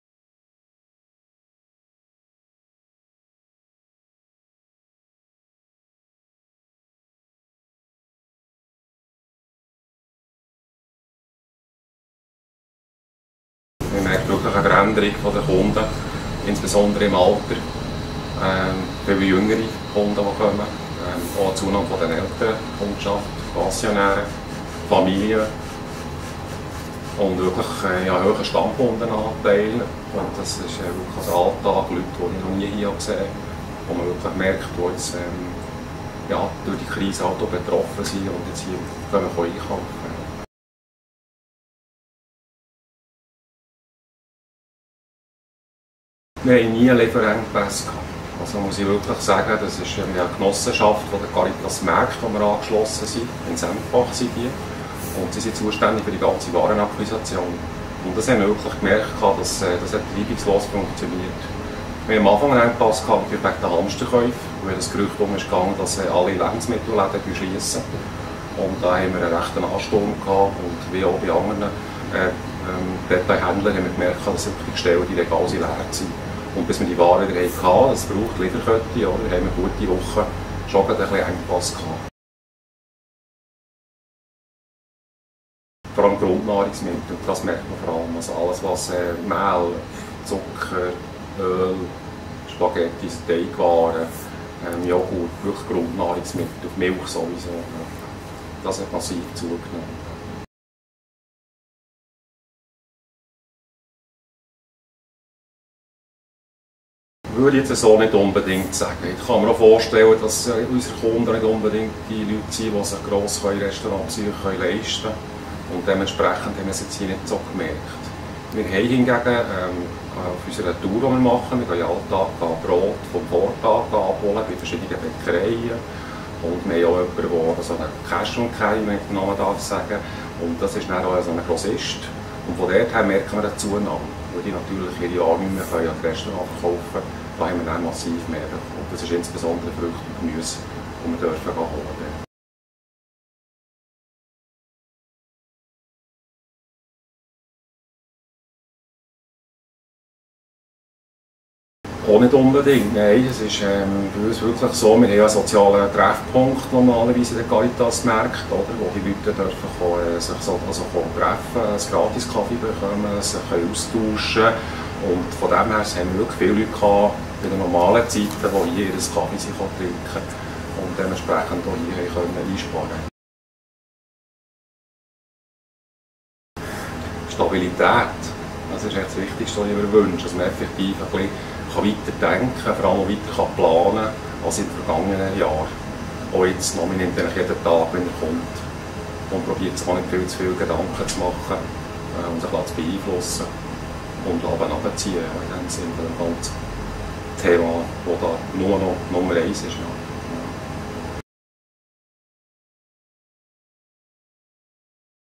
Caritas Bern: "Zu uns kommen vermehrt jüngere Kundinnen und Kunden"
Daniel Lauper, Leiter des Caritas-Ladens in Bern, erzählt über verändertes Einkaufsverhalten seit der Pandemie.